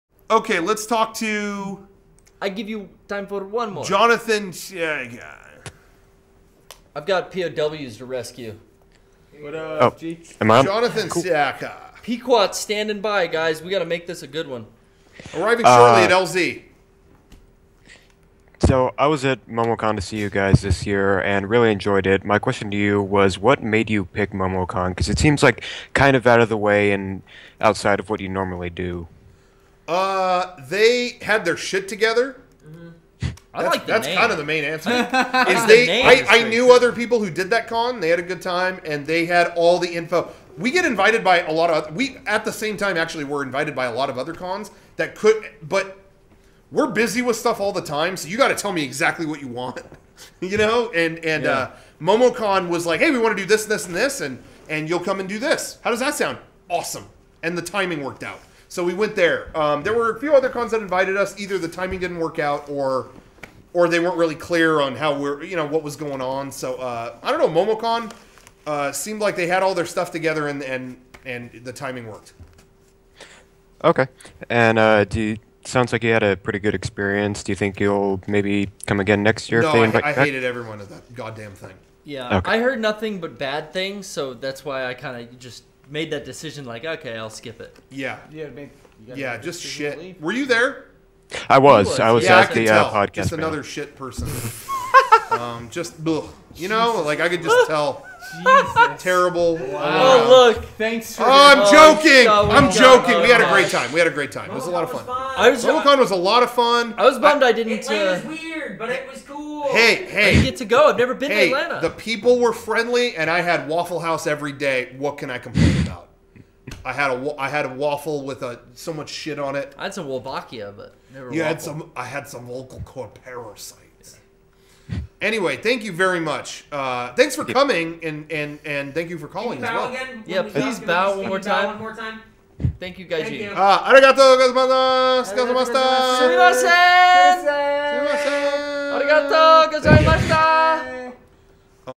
Mega64 Podcast 367 - Jonathan's Call, MomoCon 2015
Jonathan calls in to ask why Mega64 chose to attend this year's MomoCon in Atlanta, Georgia.